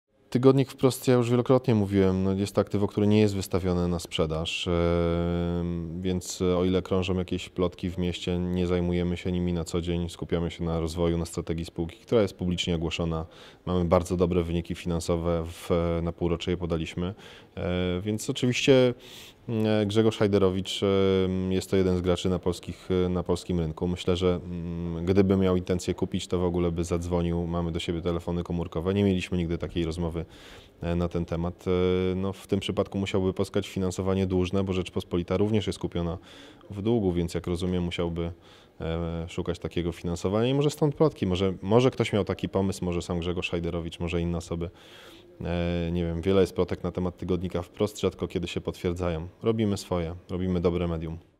Tygodnik "Wprost" nie jest na sprzedaż
Michał M. Lisiecki, prezes Platformy Mediowej Point Group SA, zapewnia, że tygodnika "Wprost" wbrew plotkom nie jest wystawiony na sprzedaż. - Grzegorz Hajdarowicz zna mój numer telefonu, ale nigdy nie rozmawialiśmy na ten temat - mówi Lisiecki.